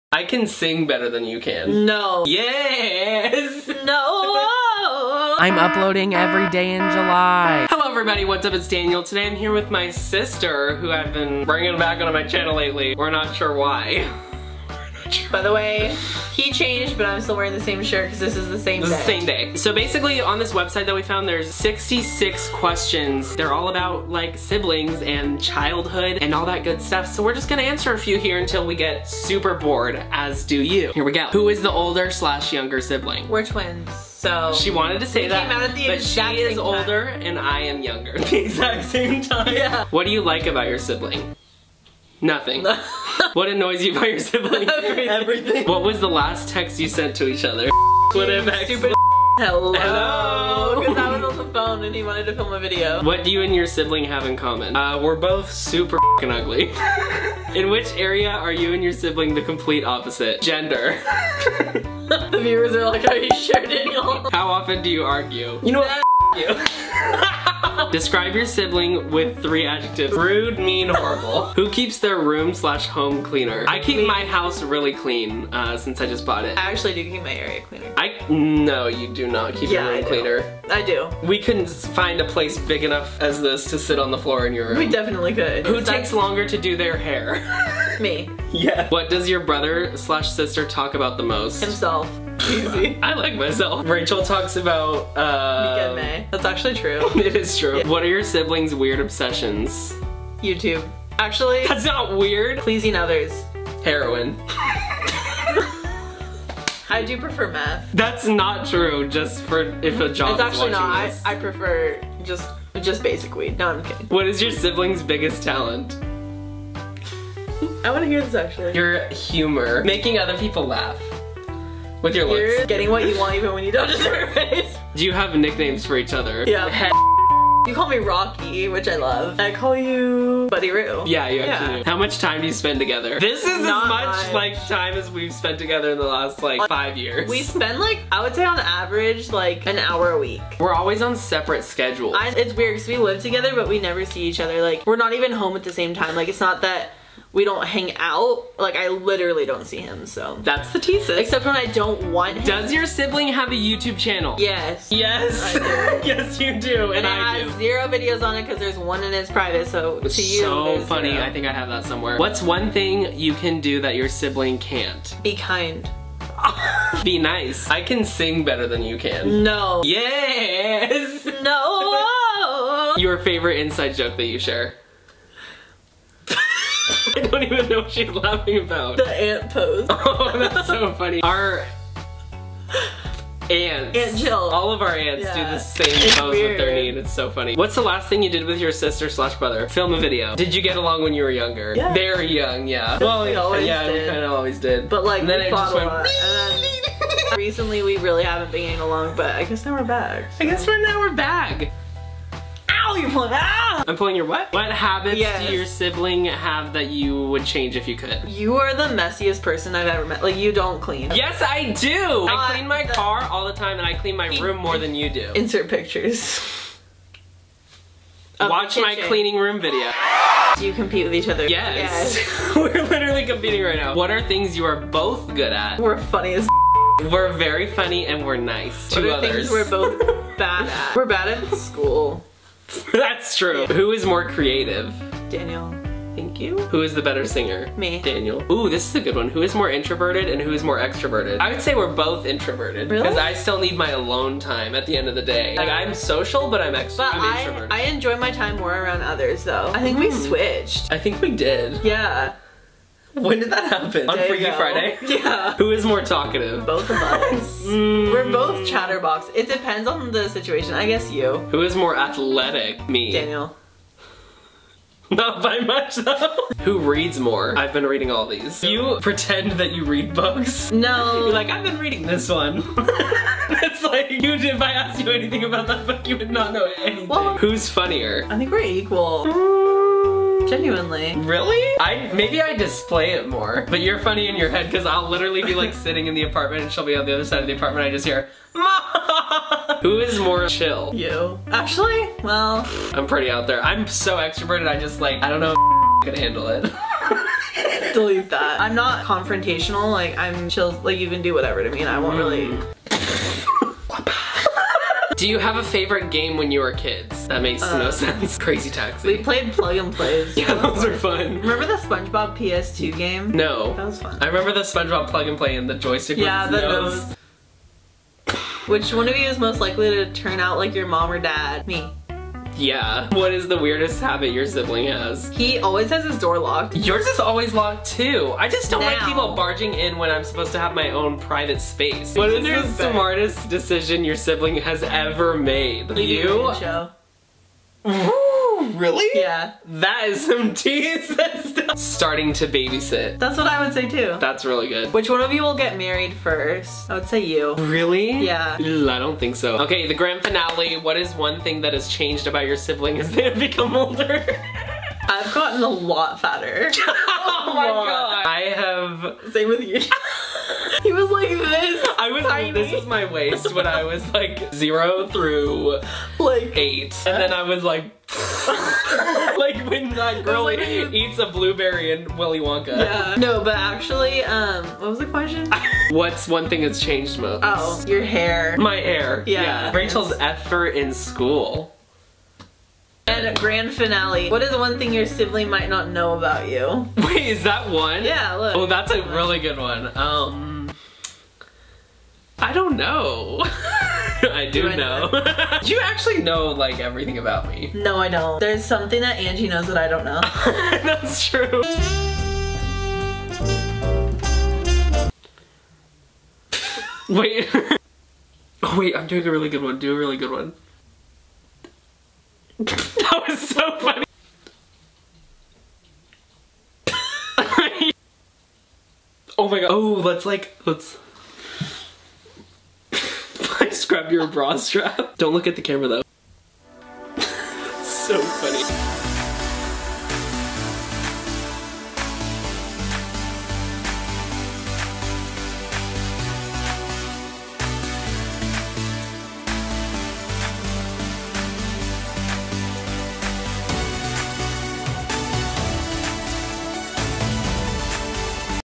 answering questions with my sister...
we faced the questions that everyone wanted answered